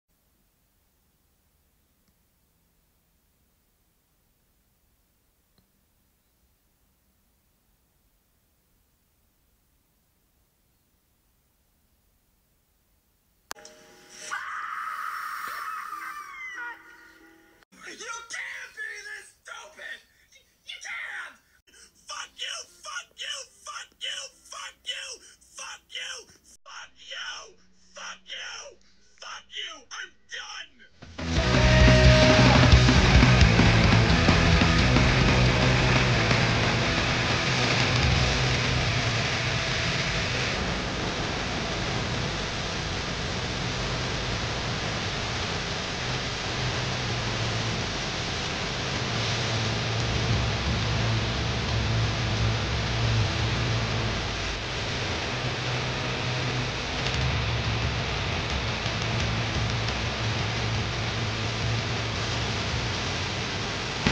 Tik Tok TEN Episode 950: I’m so done with critics!
They’ve gone too far! I absolutely REFUSE to believe that robertzemeckis new movie here is ANYWHERE this bad! Fuck critics! I’m so done with their shit! Honestly!